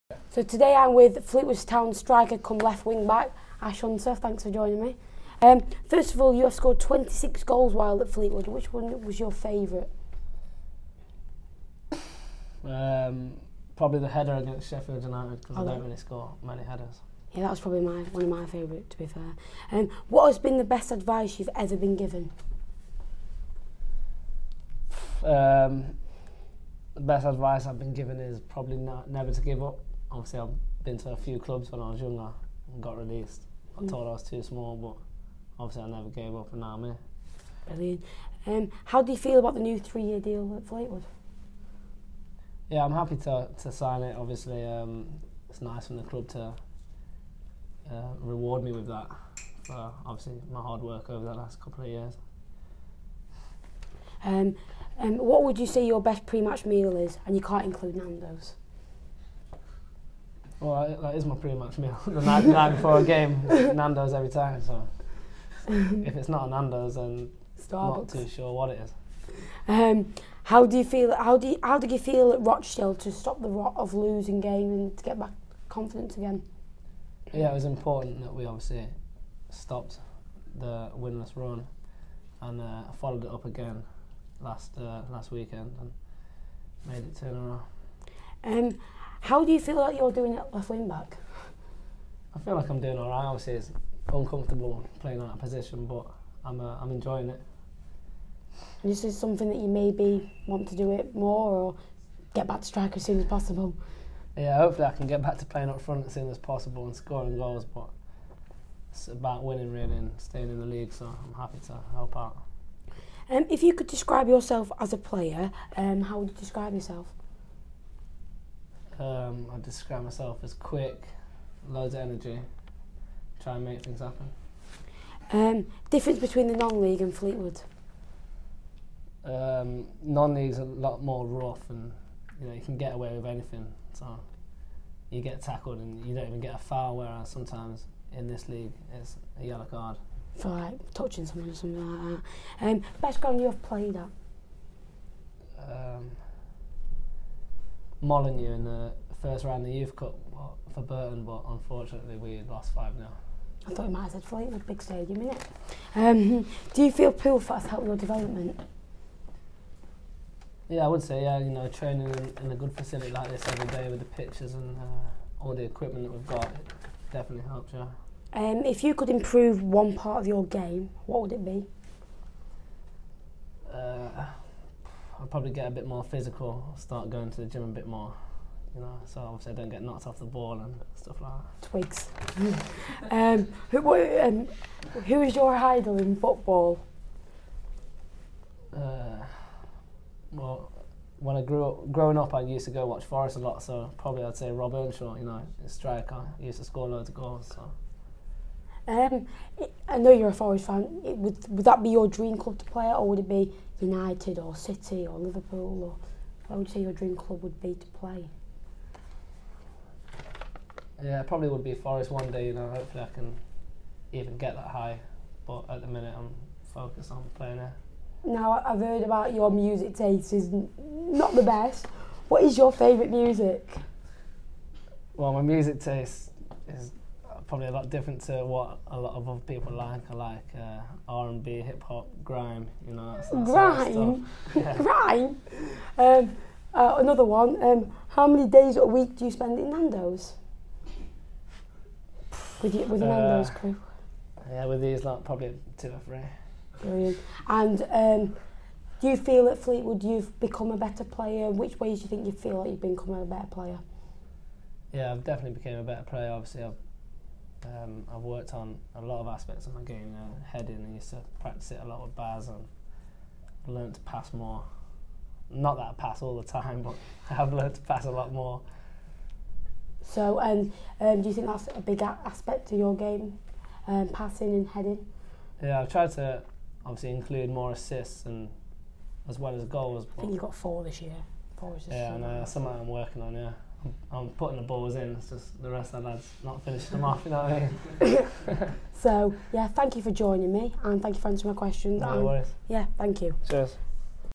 Interviewing a Professional Footballer Ash Hunter
Thank you to Ashley Hunter for taking his time out of his day to speak to me! And thank you to the club for inviting me down to interview! If you can smash a like can we hit 50 Likes?